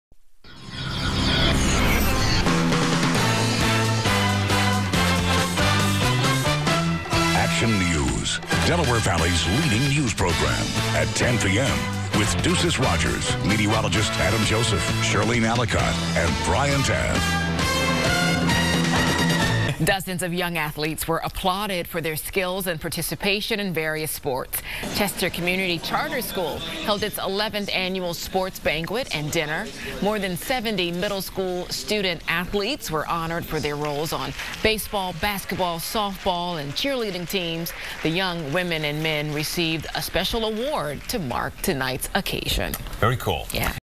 CCCS 11th Annual Sports Banquet - myPHL
Recently, Chester Community Charter School (CCCS) honored more than 70 baseball, basketball, softball, and cheerleading middle school student-athletes for their participation in the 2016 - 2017 season, at the 11th Annual Sports Banquet and Dinner, at the Clarion Hotel, in Essington, PA.

The banquet is a culmination of the season, and a celebration of the accomplishments and hard work each student-athlete applied, during the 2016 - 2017 school year. This year, the CCCS varsity boys basketball team placed second in the Penn Wood basketball tournament.